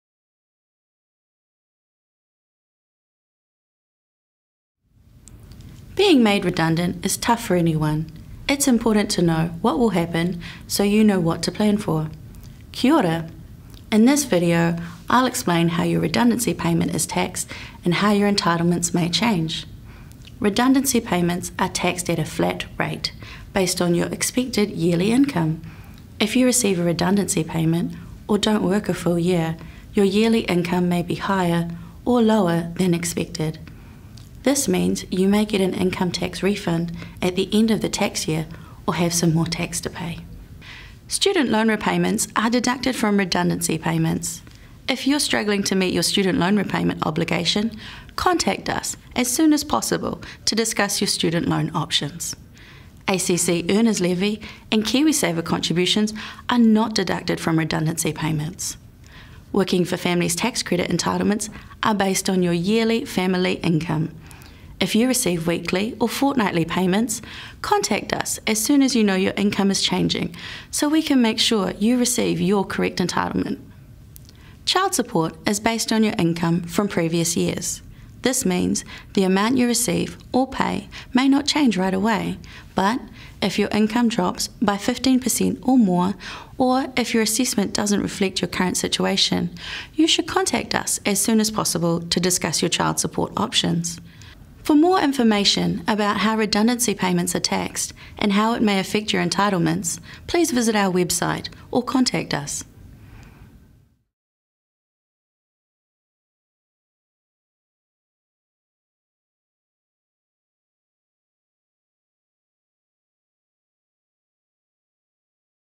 Redundancy payments | tax and entitlements
Being made redundant is tough for anyone. It's important to know what will happen so you know what to plan for. If you're going through the process of redundancy, this video will help you to understand how redundancy payment is taxed and how your entitlements may change.